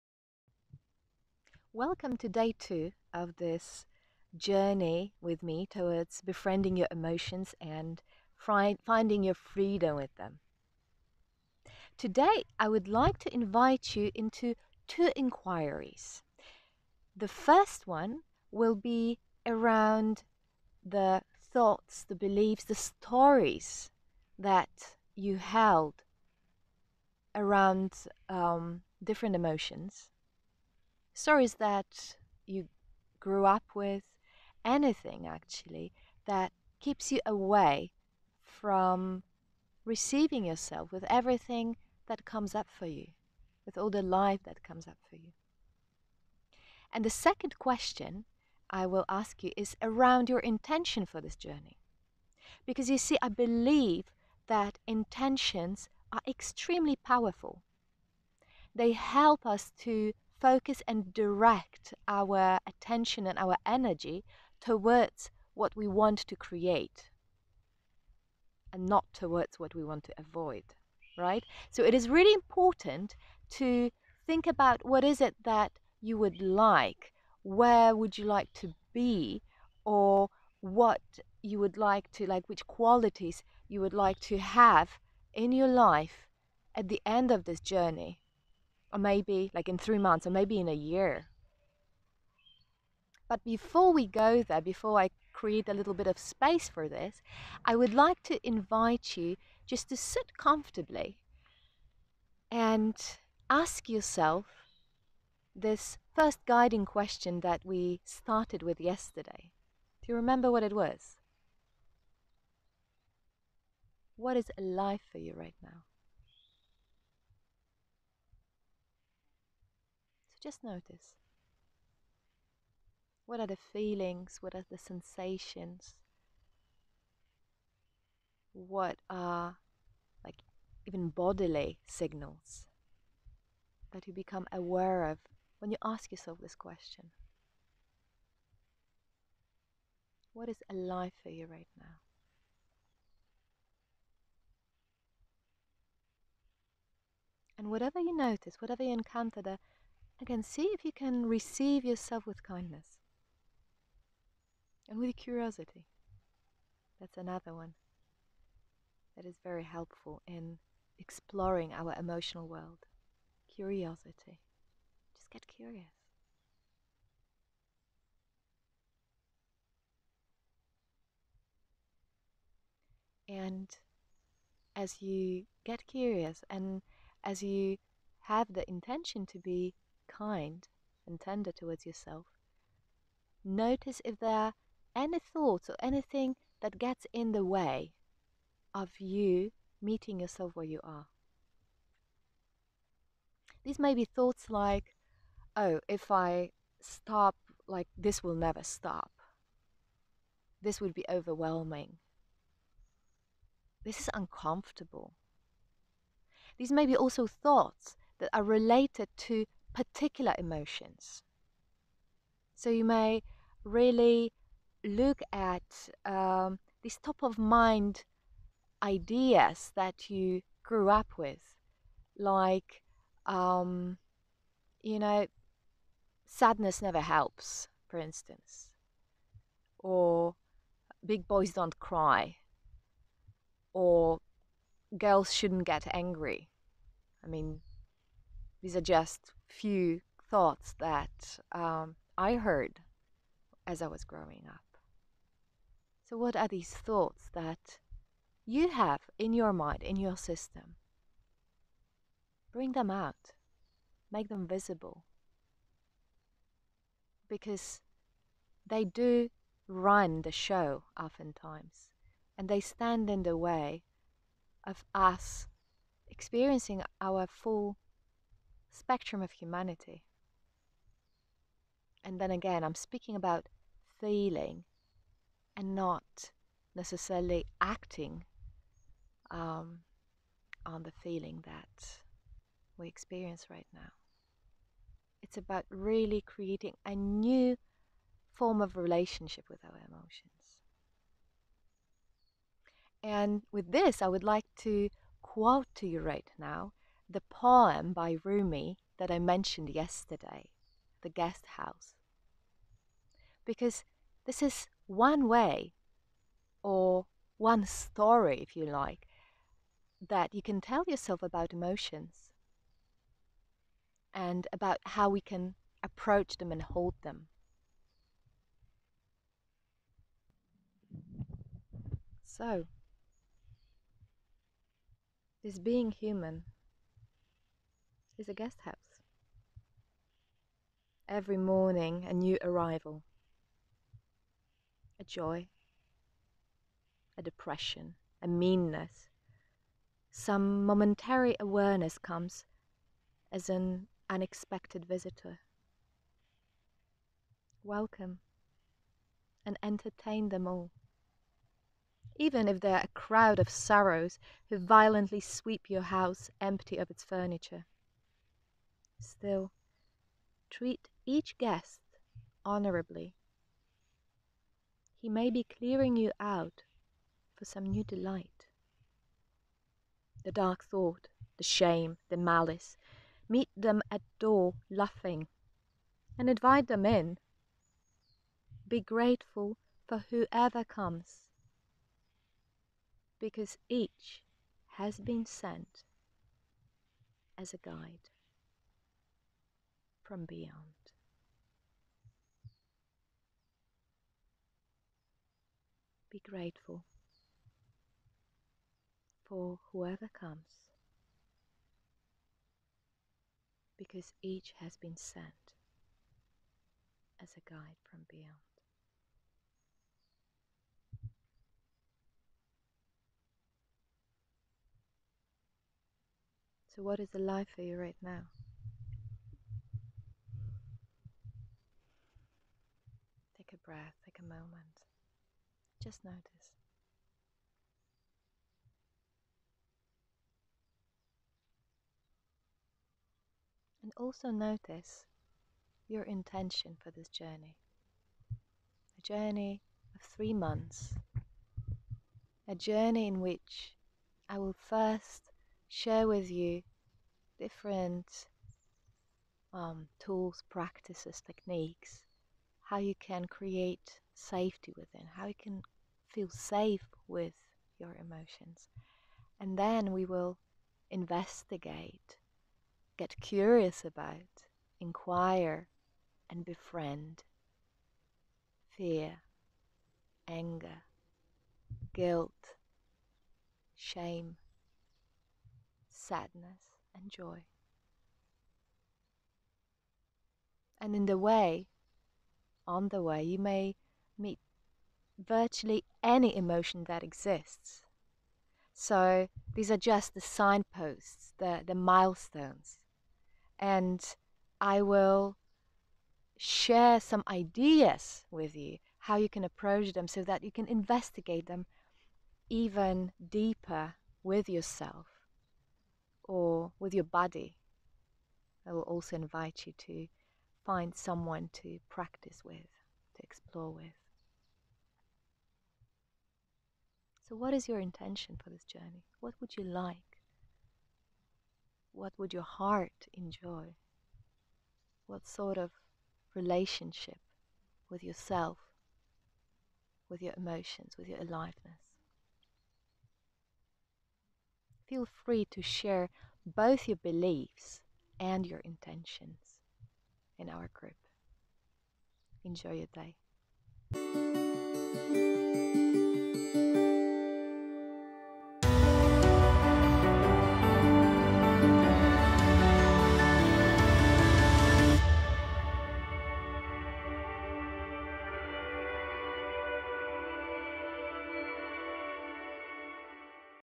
Day 2- Where are you now and where do you want to go? on your beliefs and your intention
Where are you now and where do you want to go? - on your beliefs and your intention.

This video recording is part of a 3 month long journey with Aga Rzewuska-Paca a CNVC Certified Trainer from Poland titled Free to Feel- Befriending our Emotions. If you wish to know more or be a part of this journey please